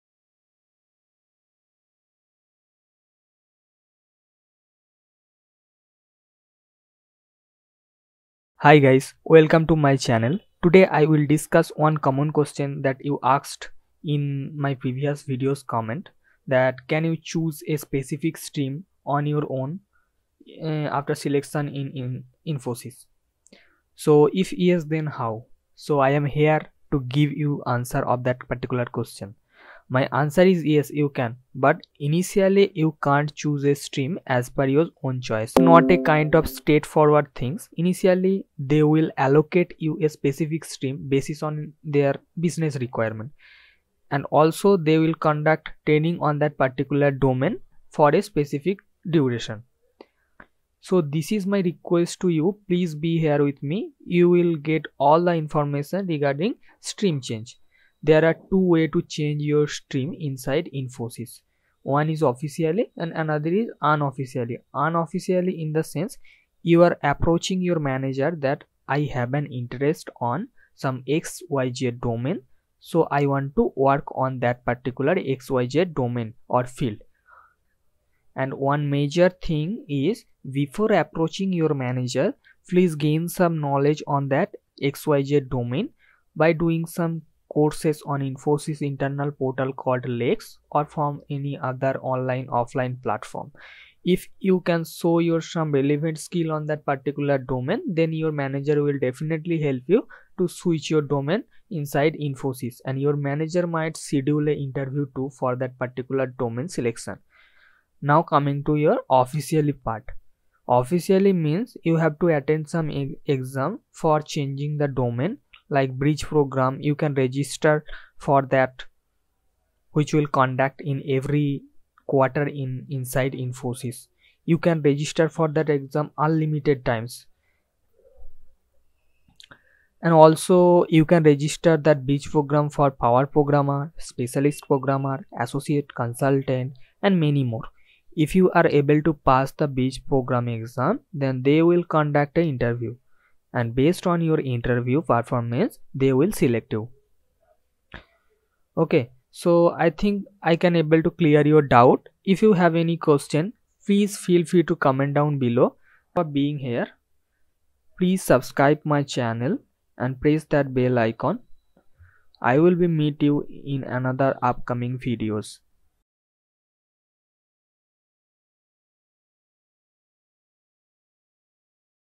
How to change stream in Infosys | Unhappy with your work? | Must watch | Useful Information🔥
Hi Guys, Today I am here to discuss stream change inside Infosys.
Infosys has a process that once you got selected, they will be allocated you to a specific stream basis on their business requirements. Happily, you have taken this one and need to complete training, and have to work on that specific stream.

Suppose you are not interested to work on that particular domain/stream which is assigned to you by Infosys, so you are in right place. Watch my video and know about the process and start work on it. Best wishes from me.

If you like the video please keep supporting me.